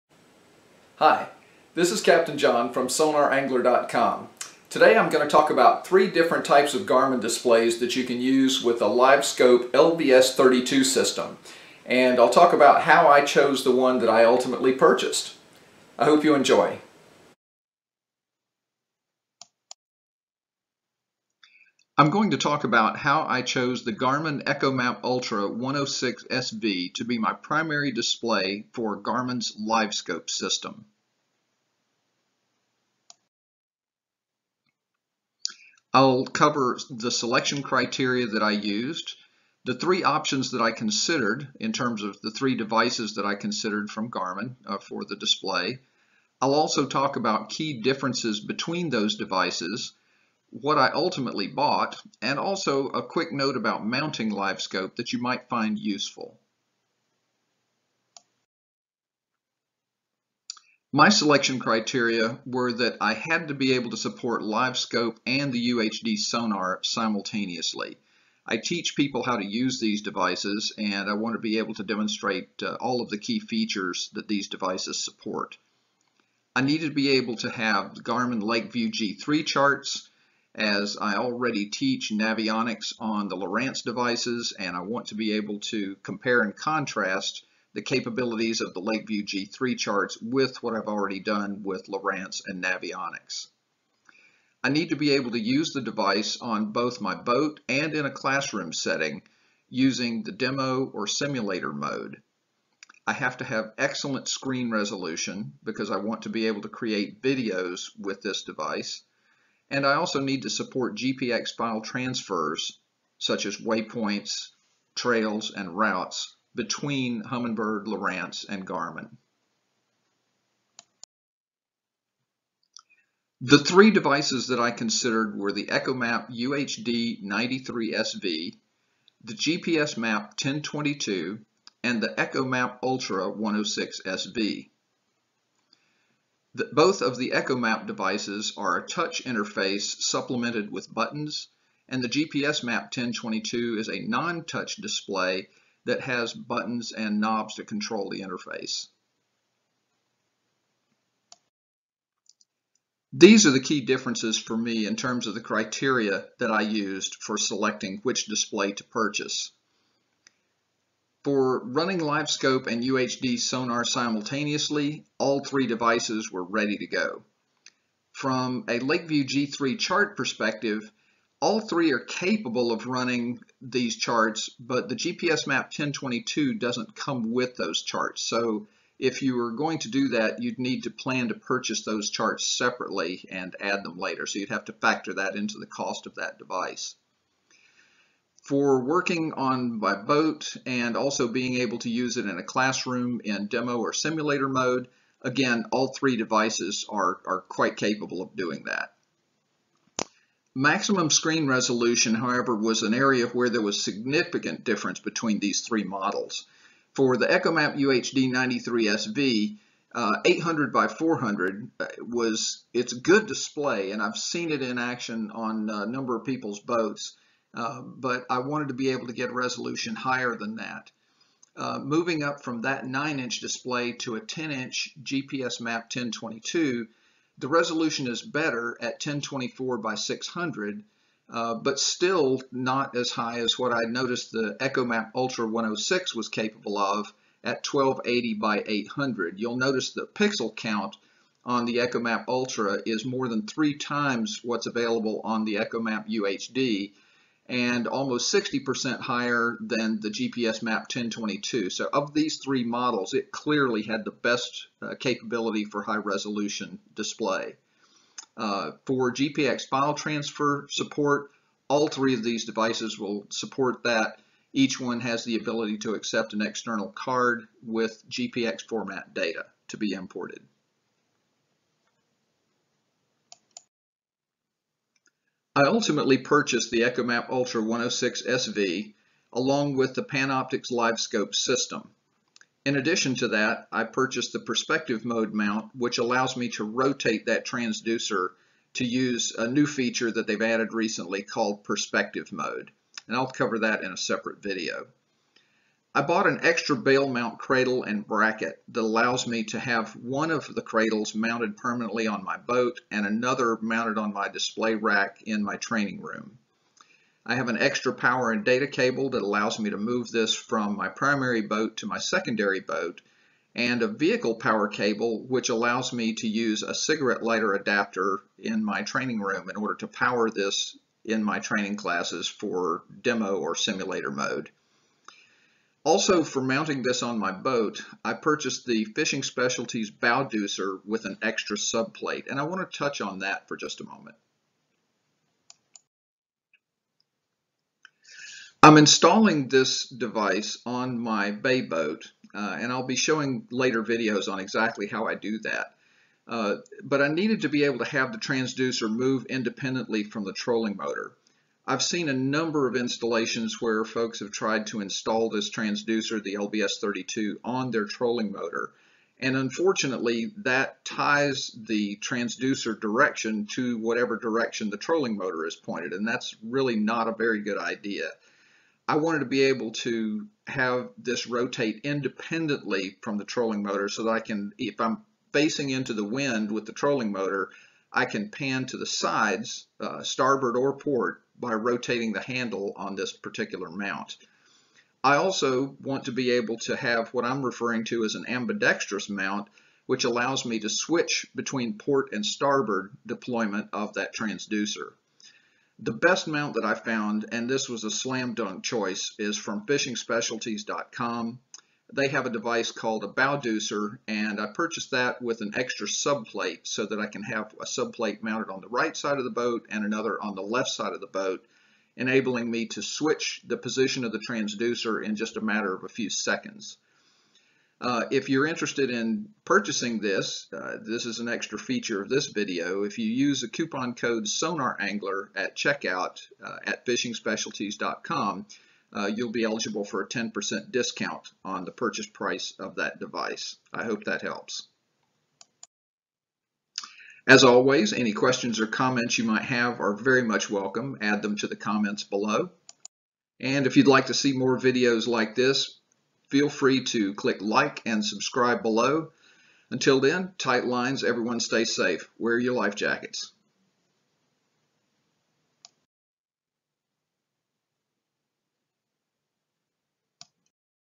LiveScope and Echomap Ultra 106sv Part 1
This is the first part of a series about my selection and installation of Garmin's LiveScope System LVS32, and Echomap Ultra 106sv display.